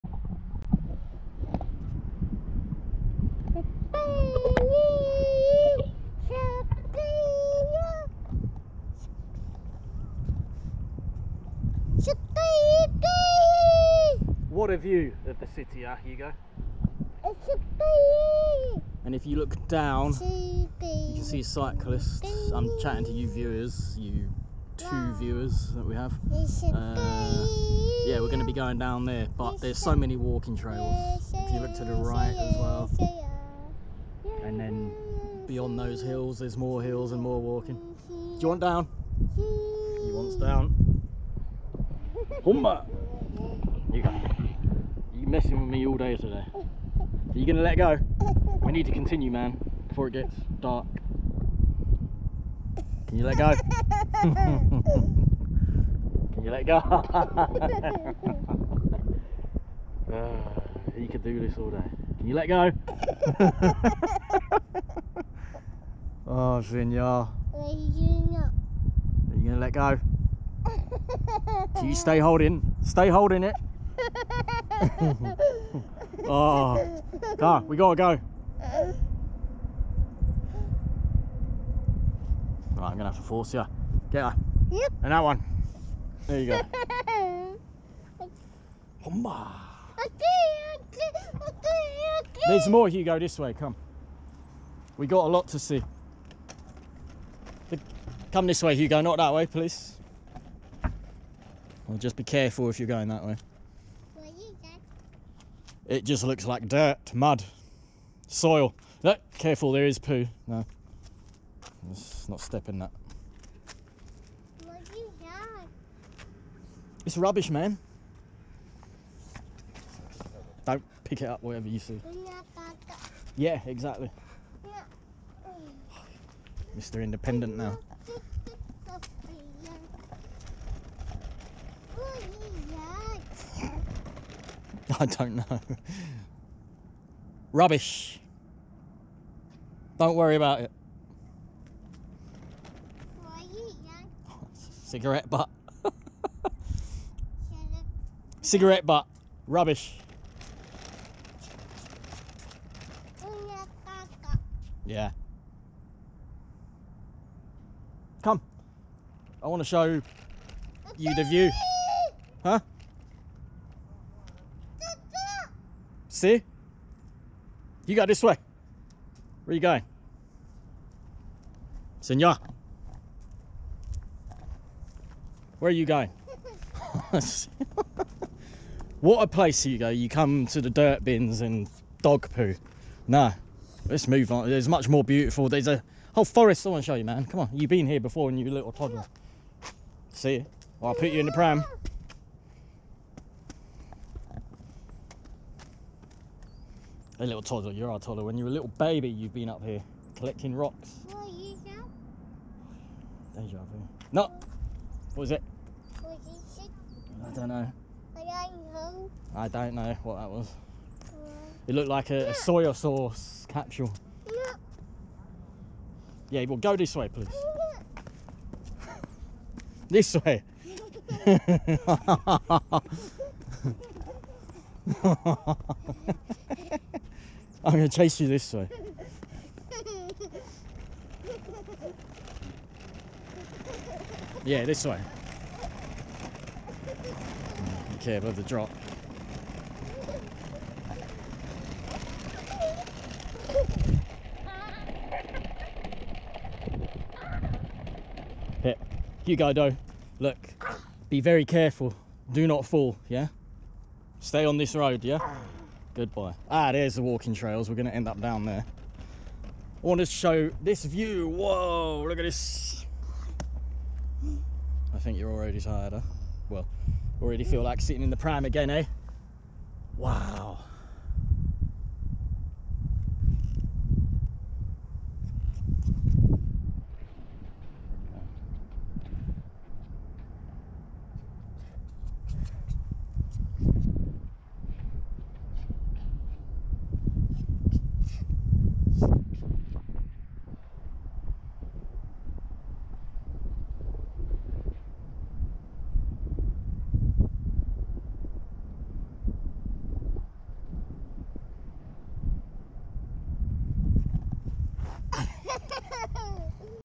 Father and son travel memories. an adventure into Barcelona's mountain towns and nature trails. 360
Surrounding exciting Barcelona City like a protective shield are the mountains of Barcelona. The city  with its sexy cosmopolitan districts and vibrant trendy alleyways in its gothic centre has a diverse range of acitivites and landscapes for the outdoor and nature lovers...

Beaches, parks, nature trails, mountains and forests are all to be discovered a short distance away from the main city attractions.

Me and my boy Hugo took a lovely day trip up into the surrounding mountains, viewing points and walking routes.

We only skimmed the surface of what these miles of mountains  have to offer and I filmed in 360 with my go pro fusion that has scratches on both lenses...I plan to upgrade to a new camera but I am enjoying the lofi aspect of the go pro fusion now that its a few years old..